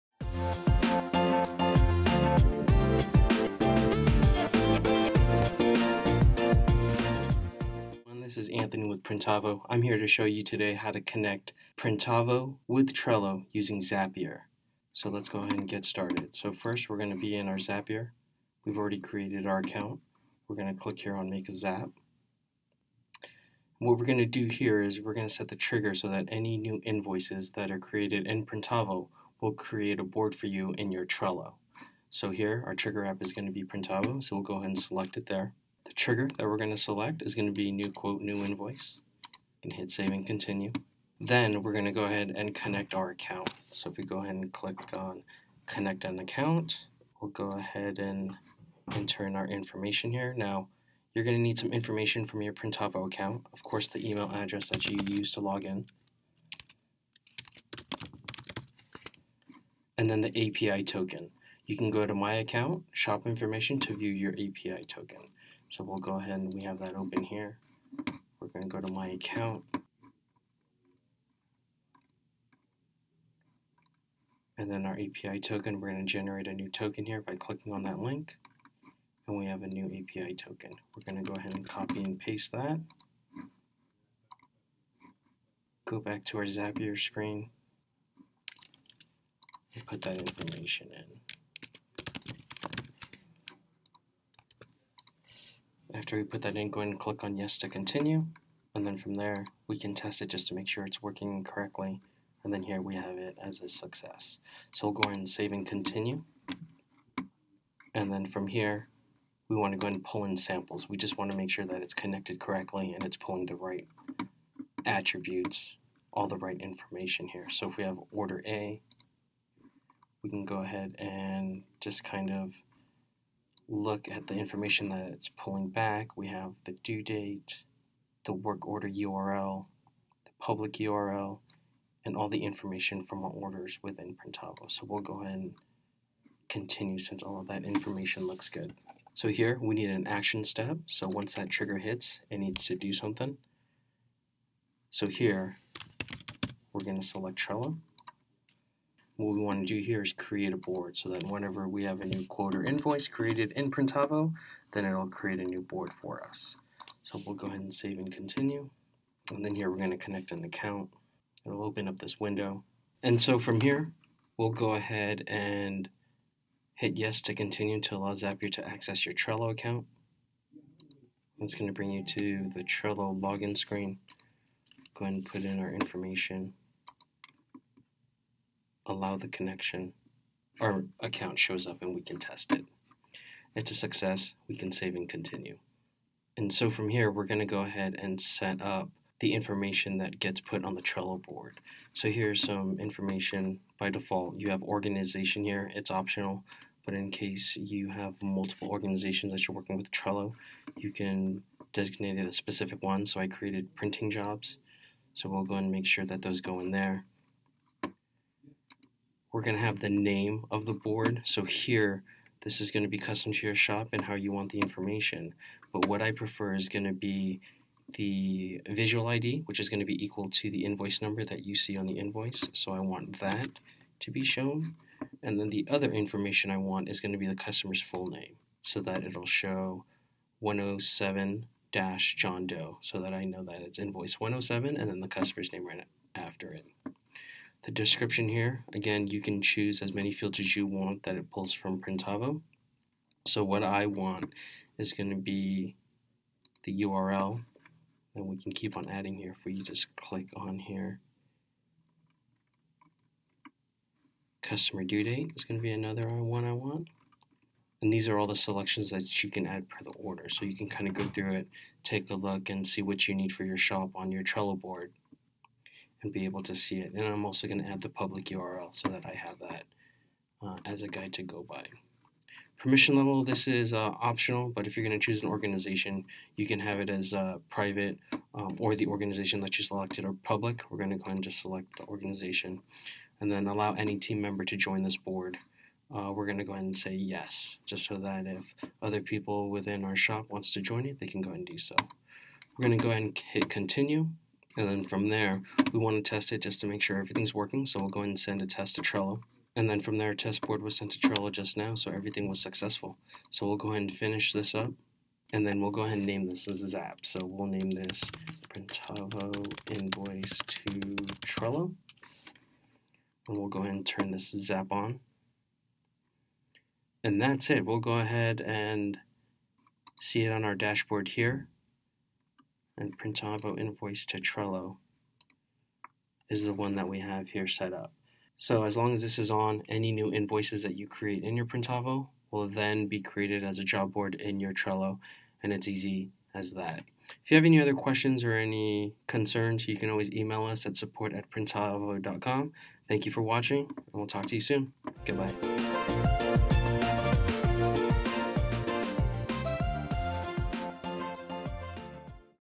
Connecting Printavo to Trello using Zapier
In this video, we will go through how to create a Zap so a new Quote/Invoice created in your Printavo will create a new board in Trello.

To be able to do this you are going to need a few things. You need to have a premium subscription to Printavo. Also, you are going to need a Zapier account already set up as well as a Trello account.

Log into Zapier and create a Zap

The trigger App is going to be Printavo. Once you select that, the New Quote/Invoice is going to be the trigger you want to select.

From there connect your Printavo account. You are going to need your email you log into printavo with and your API Token. Your API Token can be found here and scrolling down:

After you successfully connect your account, you can test it by pulling in samples, reviewing it and making sure it is correct. After it all checks out, then add the second step.

Select the action app of Trello. Then from there, select to create Create Board under the Trello action. You are going to then connect your Trello account by using your login information. Once you test and get a successful connection, you can continue to set-up what information will go where.

At this point, this is where you get to decide what information will go where. In the video, I chose very simple information, but when you set it up, you can decide how much information you include in each of those fields.

Once you choose, you can continue and test the zap to make sure it works. Once you get a successful test, then you can turn on your zap and it will be ready to go.